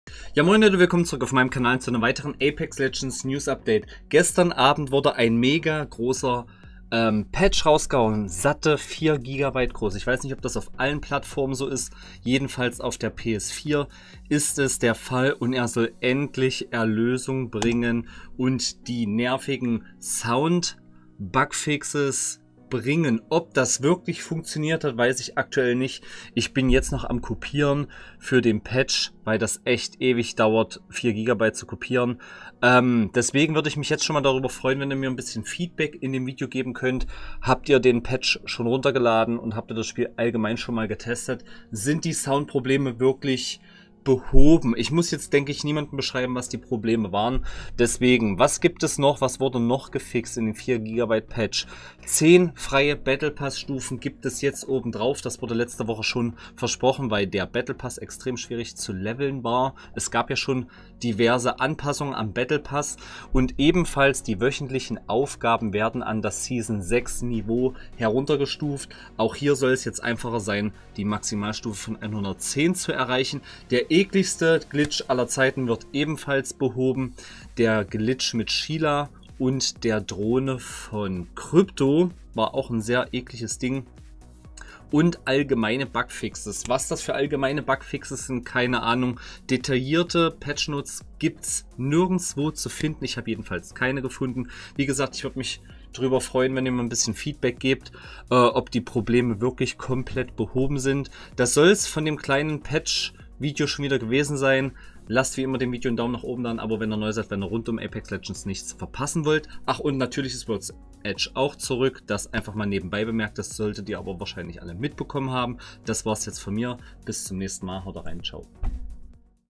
Patch 1.53 bringt Soundfixes , Battlpassänderungen und Bugfixes│Apex Legends Season 7 deutsch
4GB ist der neue Patch in Apex Legends Season 7 groß und bring uns die erhofften Soundfixes , Änderungen am Battlepass und der heftigste Glitch mit Rampart und der Cryptodrohne wurde ebenfals behoben!